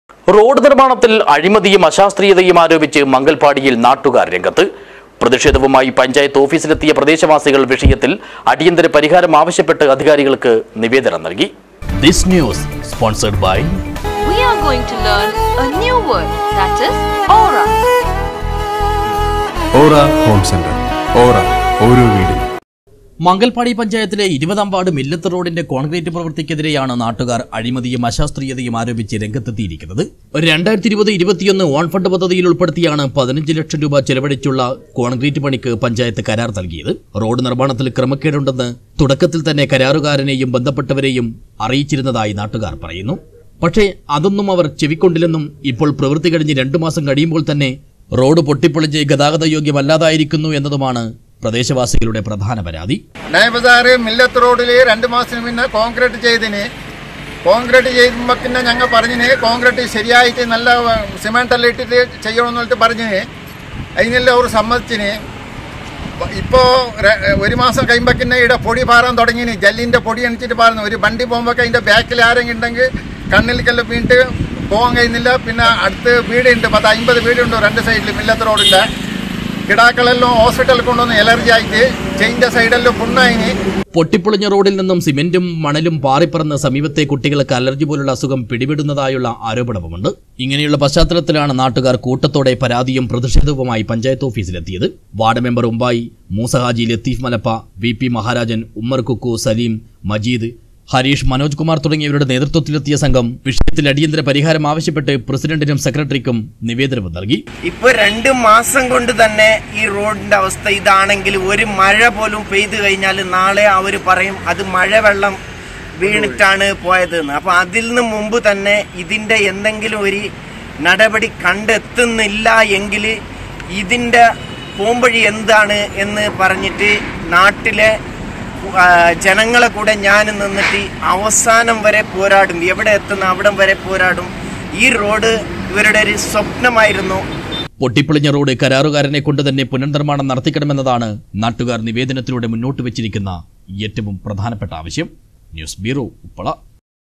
റോഡ് നിര്‍മാണത്തില്‍ അഴിമതിയും ആശാസ്ത്രീയതയും ആരോപിച്ച് മംഗല്‍പാടിയില്‍ നാട്ടുകാര്‍ രംഗത്ത്.
റോഡ് നിര്‍മാണത്തില്‍ അഴിമതിയും ആശാസ്ത്രീയതയും ആരോപിച്ച് മംഗല്‍പാടിയില്‍ നാട്ടുകാര്‍ രംഗത്ത്. പ്രതിഷേധവുമായി പഞ്ചായത്ത് ഓഫീസിലെത്തിയ പ്രദേശവാസികള്‍ വിഷയത്തില്‍ പരിഹാരം ആവശ്യപ്പെട്ട് അധികാരികള്‍ക്ക് നിവേദനം നല്‍കി.